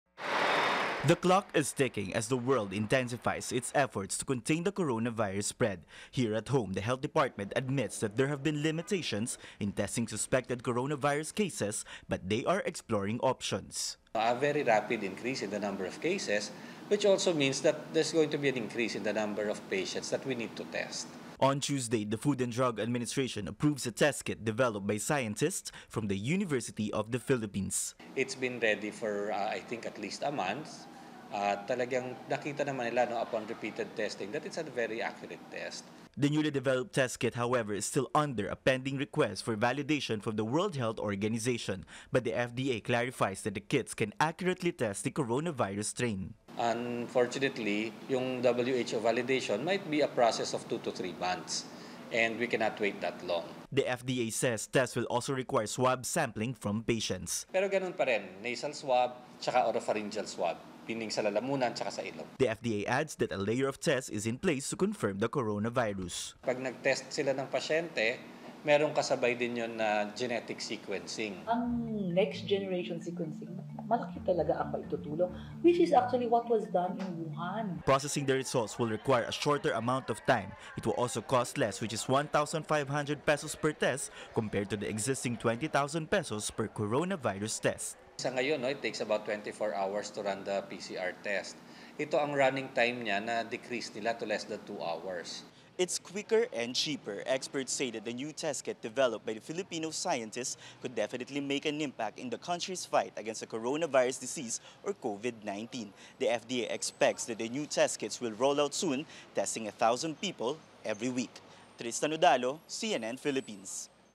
FDA approves coronavirus test kit developed by Filipino scientists
Filipino scientists develop a coronavirus test kit that produces results in a short period. The more affordable kit is expected to be out in the market soon.